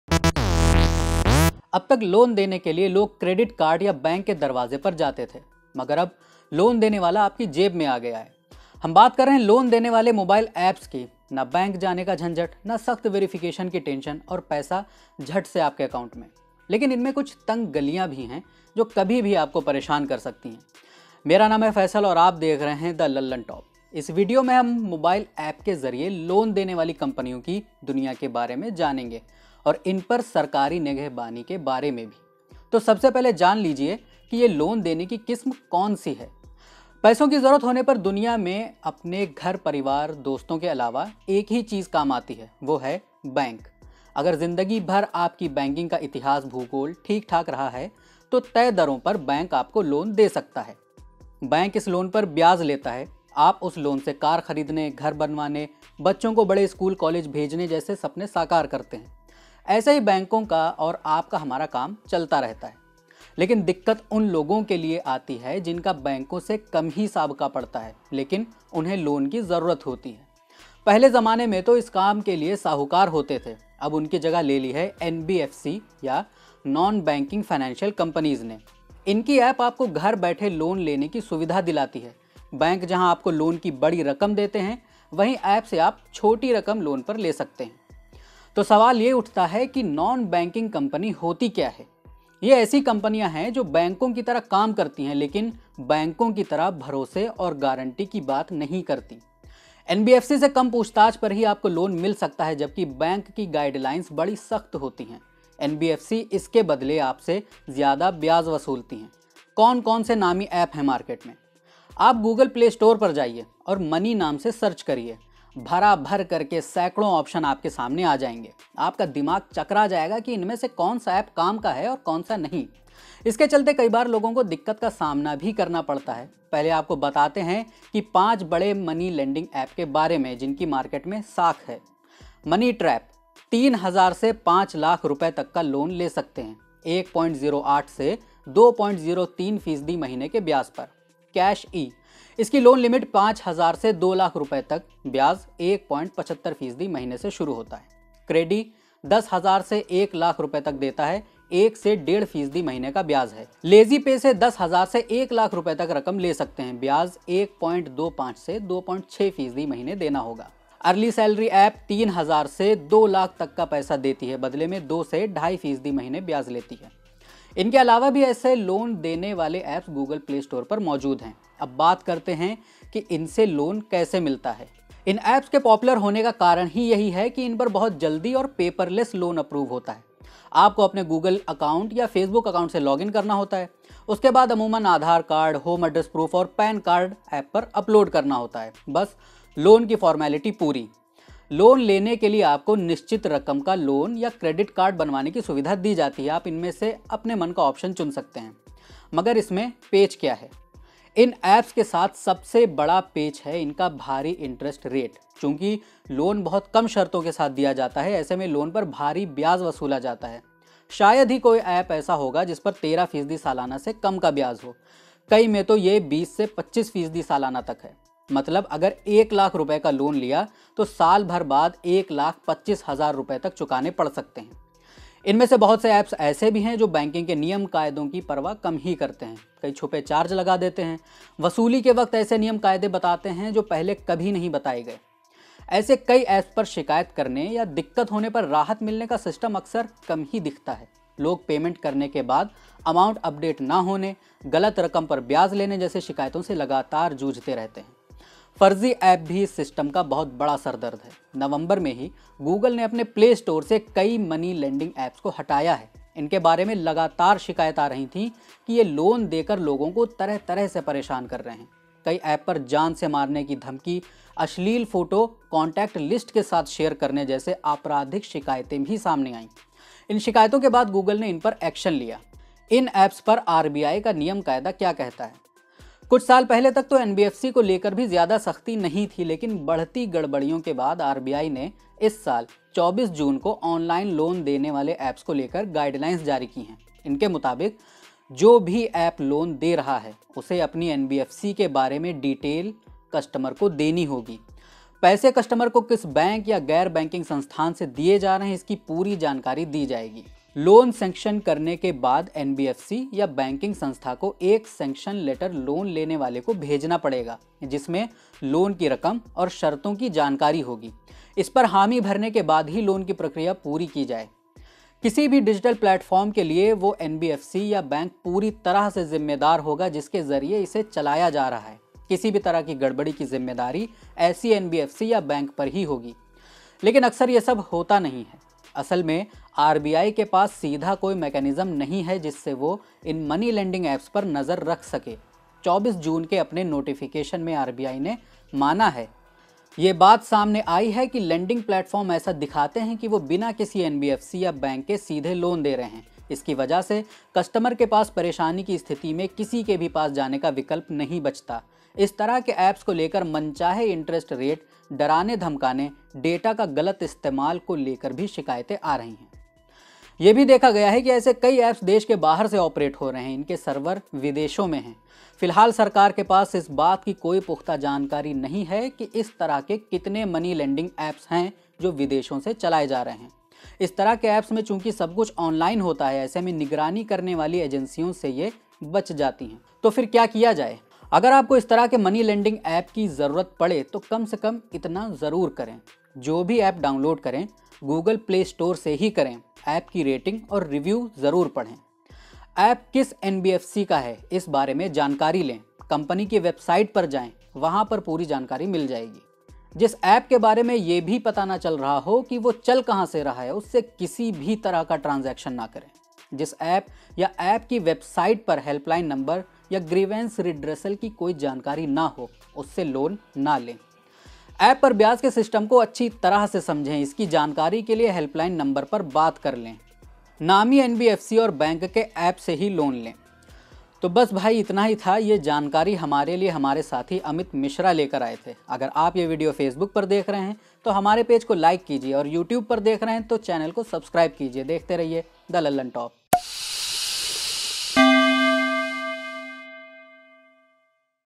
फटाफट लोन देने वाली Money lending apps की मनमानी को क्यों नहीं रोक पा रही RBI?
A crop of new-age financial companies is pushing young people into massive debts. This is what happened in the case of 23-year-old Sai Aravind, an IT professional from Chennai, who died by suicide unable to cope with pressure exerted by money lenders app. What are these money lender apps and why it's tough to control them? Even RBI is feeling helpless.